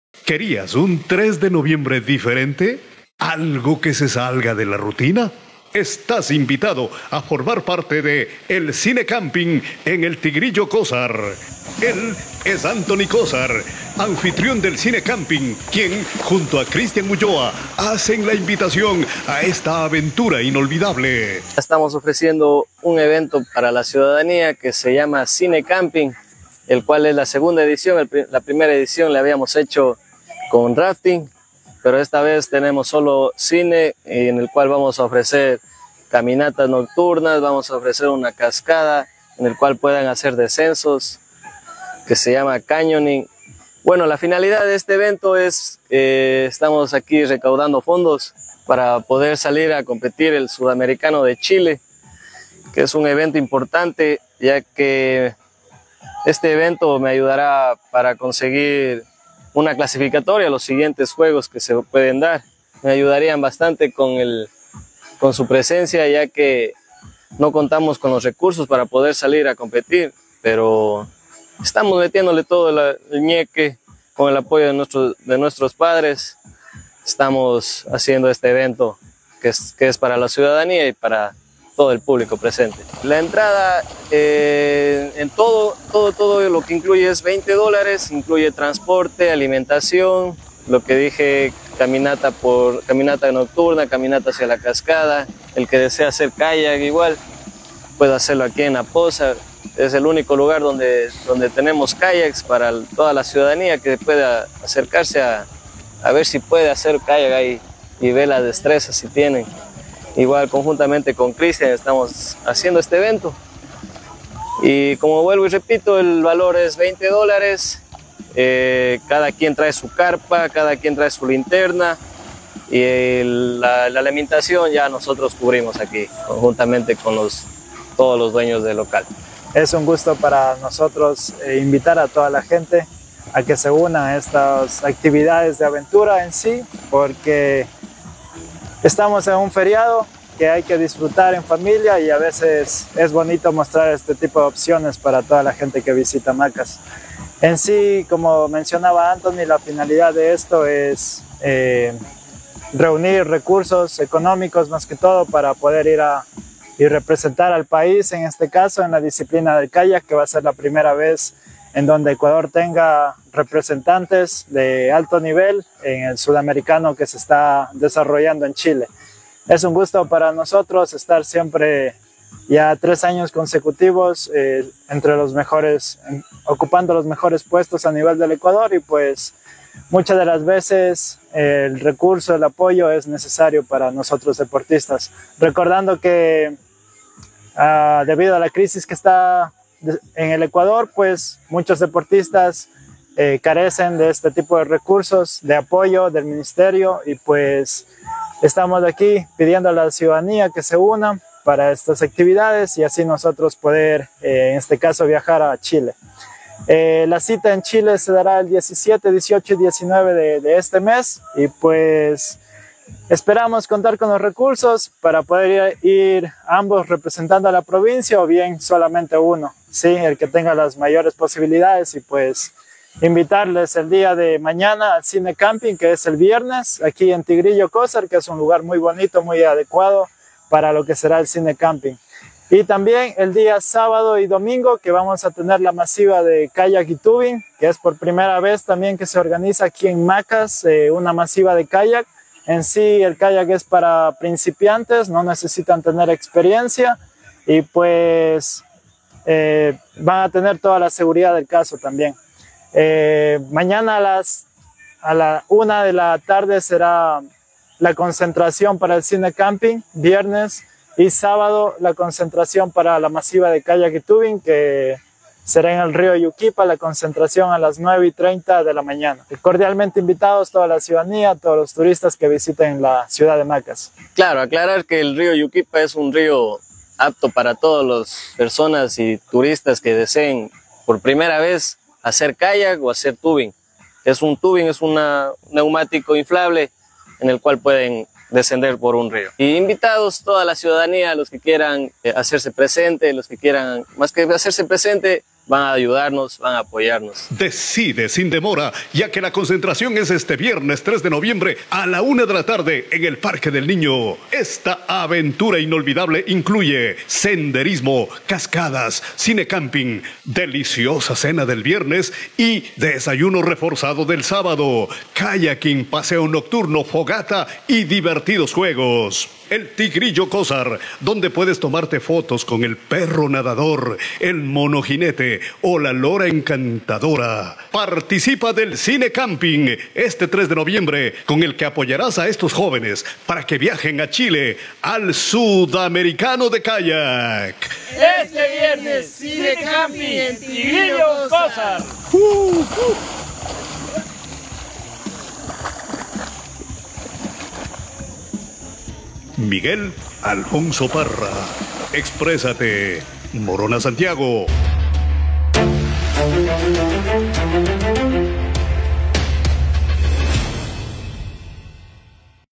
Prepara tu carpa para el "Cine Camping" esta noche en el Tigrillo Cózar
[TURISMO] EVENTOS: MoronaSantiago ¡Sal de la rutina haciendo algo diferente!.. Esta noche usted puede formar parte del "Cine Camping" en el Resort Tigrillo Cózar ubicado al fondo del barrio Lenin Moreno de la ciudad de Macas.

El evento incluye: Transporte desde el parque del niño a las 13h00 de hoy viernes 3 de noviembre, deliciosa cena campestre, desayuno reforzado del sábado, senderismo, cascadas, descenso en cuerdas, divertidos juegos y muchas sorpresas.

Ven con tu carpa y tus implementos personales a una experiencia inolvidable. Costo 20 dólares por persona

Mas información al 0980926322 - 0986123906